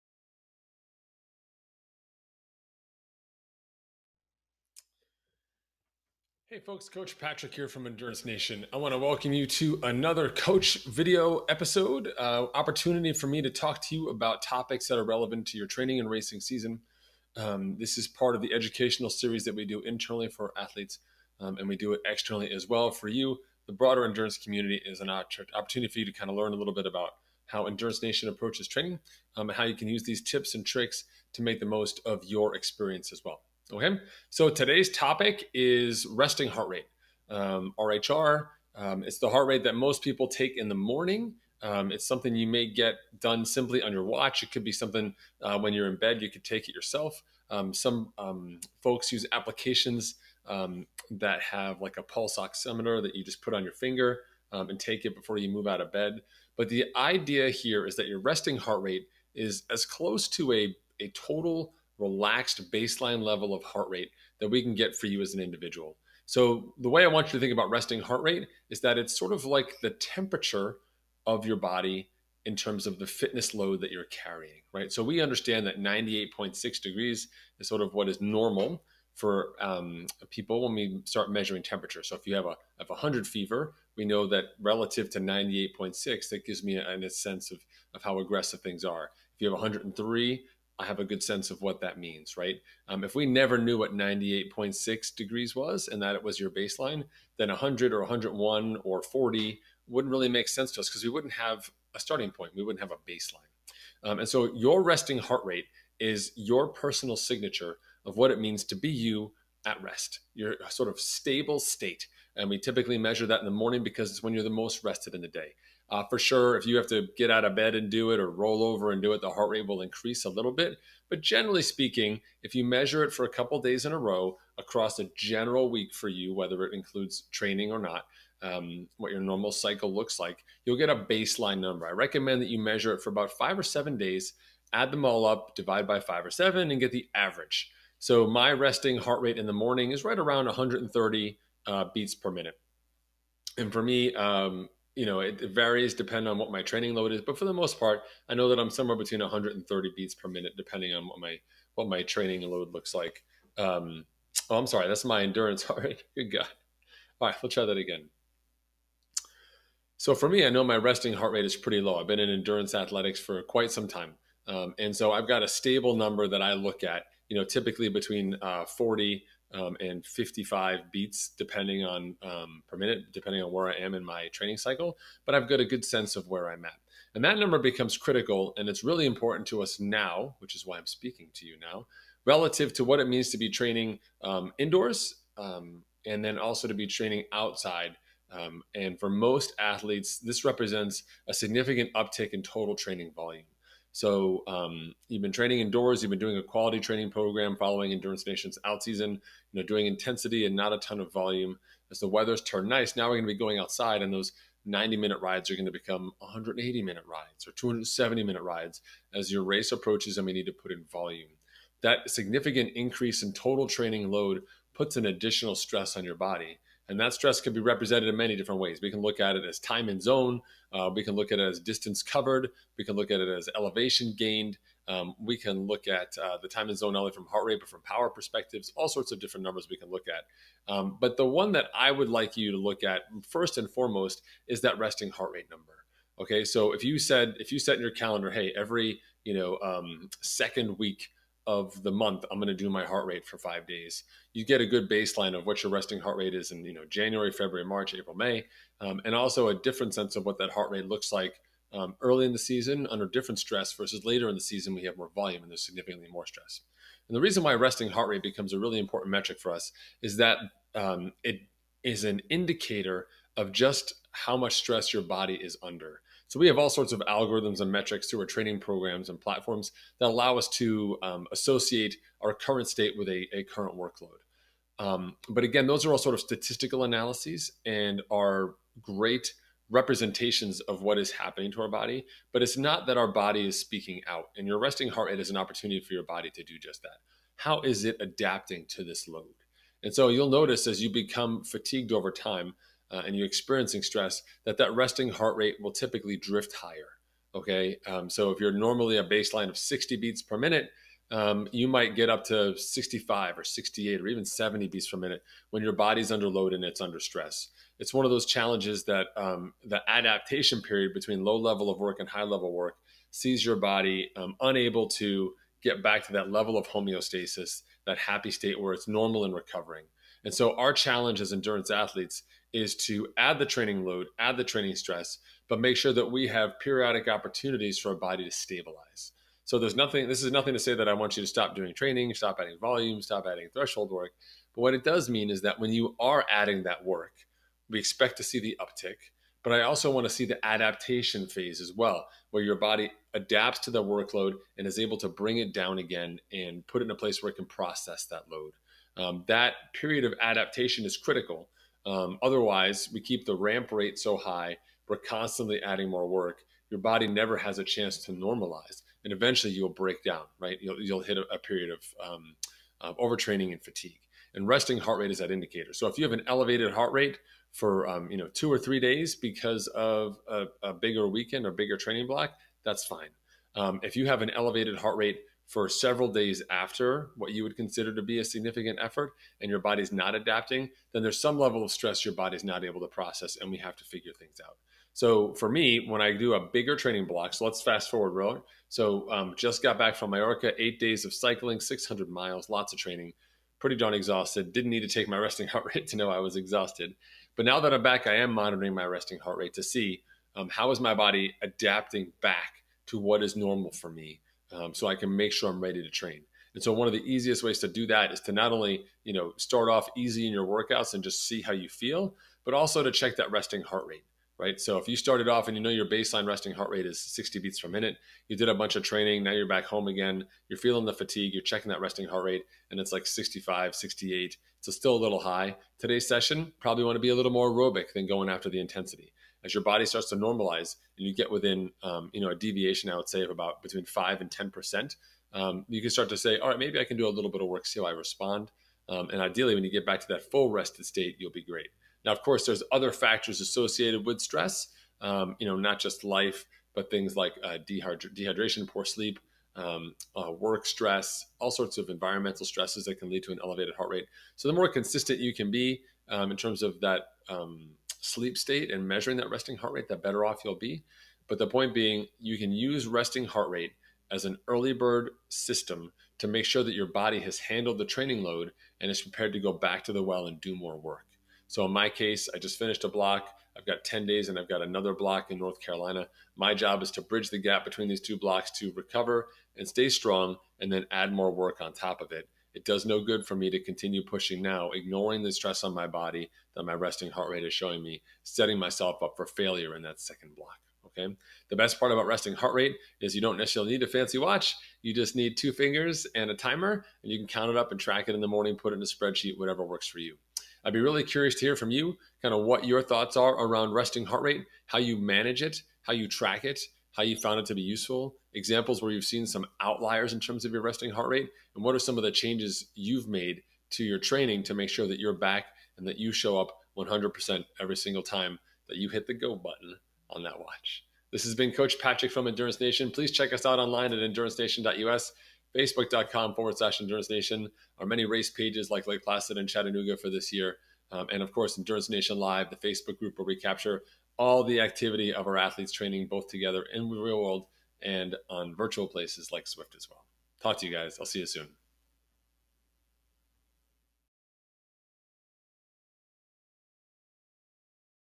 Using Resting Heart Rate to Manage Early Season Fatigue
for this mini lesson on the value of Resting Heart Rate (RHR).  You likely use some form of software to track your training, but few of us have a visceral connection to that data. Tracking your RHR is a chance to get a peek as to how your body is handling your training and the transition to more volume (or whatever your next phase might be).